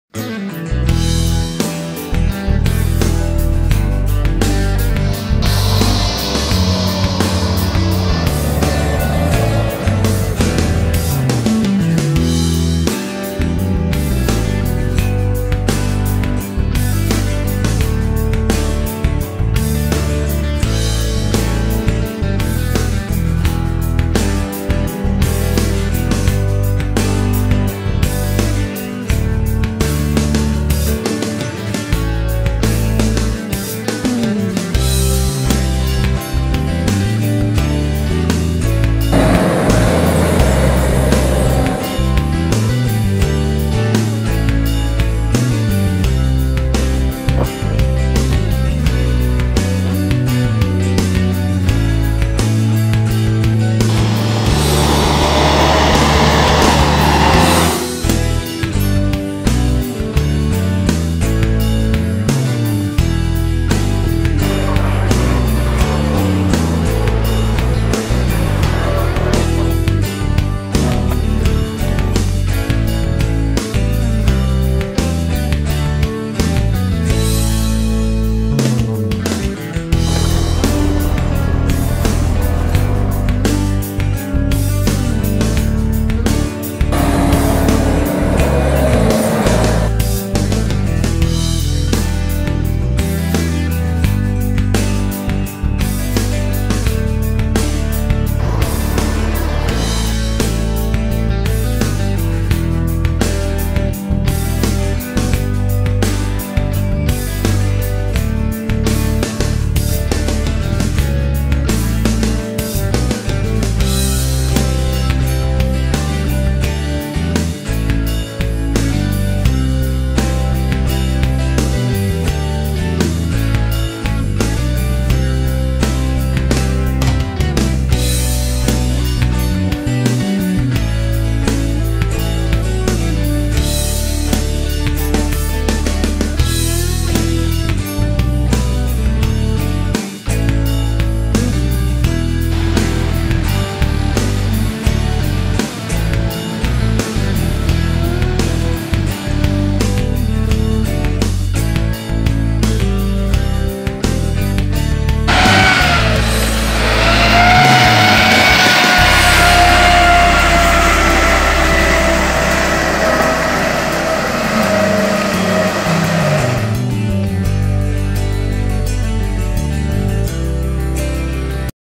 Ramp Country Team z AMATOR 4x4 Piaskowa Pułapka & Jura Park
Relacja z wyjazdu z AMATOR 4x4 na wyprawy Piaskowa Pułapka i Jura Park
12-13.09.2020